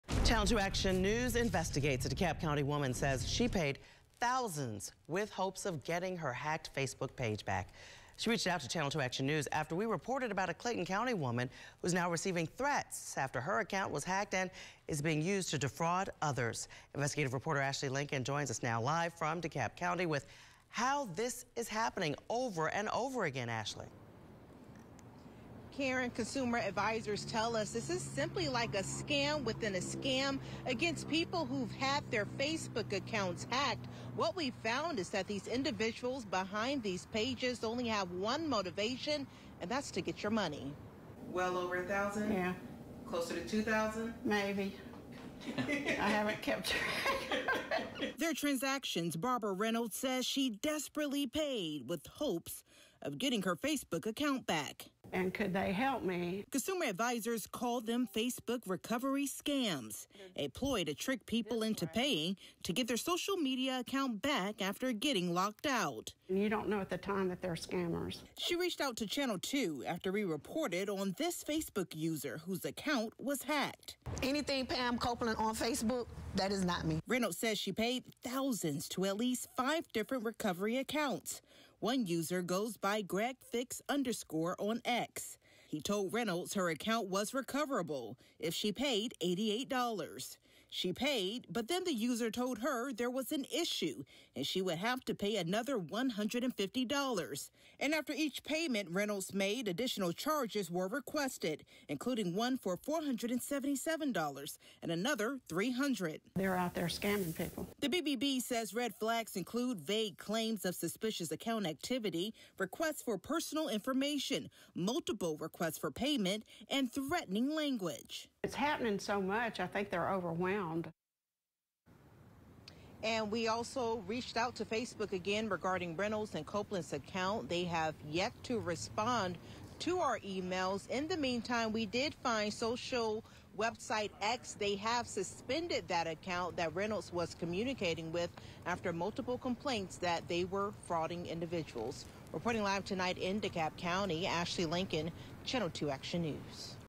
Facebook account hack in Clayton County leaves resident desperate for help
A DeKalb County woman said she’s paid thousands of dollars to a hacker with the hope of getting her Facebook account back.